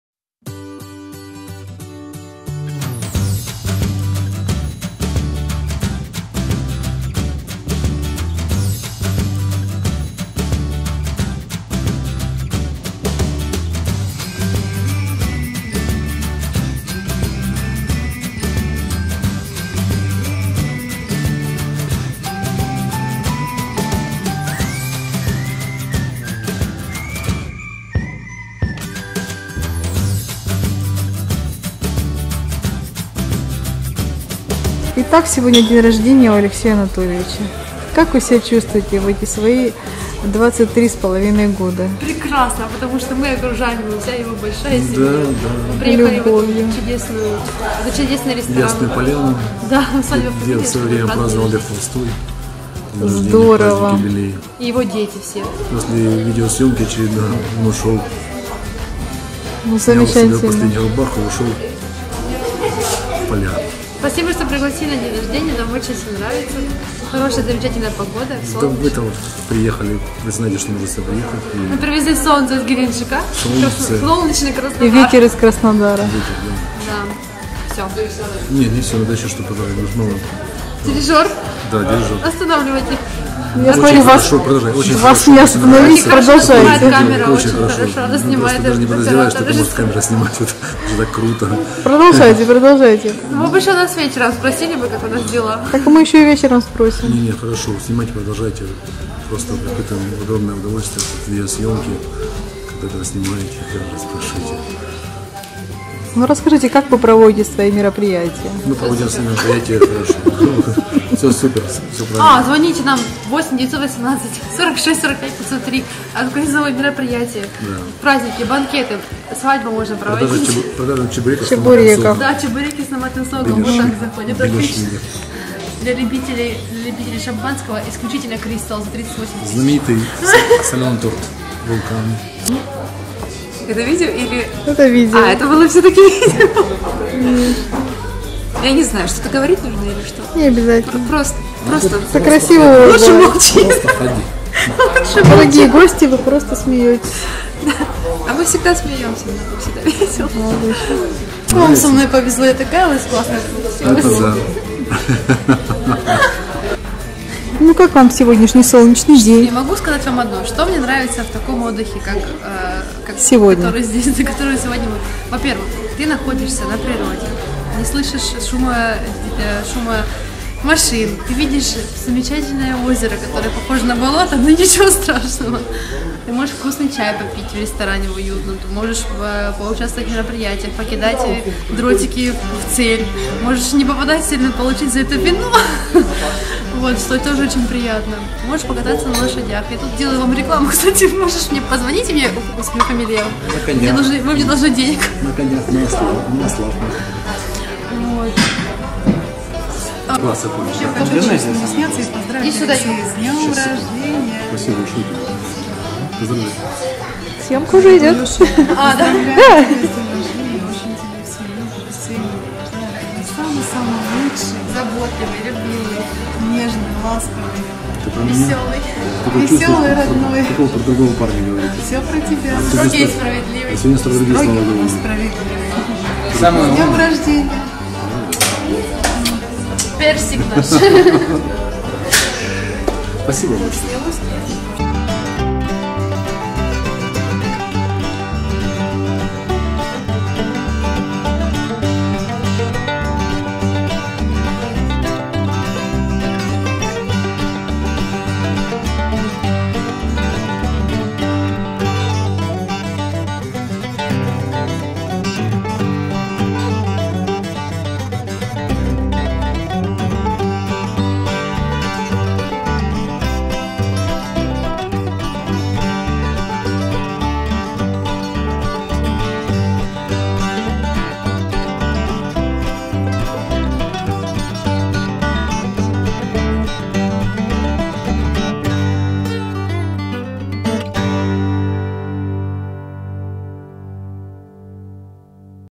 УСАДЬБА ФАМИЛИЯ. ОТДЫХ НА ПРИРОДЕ.
Семейный выезд на природу 31 марта 2019 года.Место -Краснодарский край, станица Пластуновская .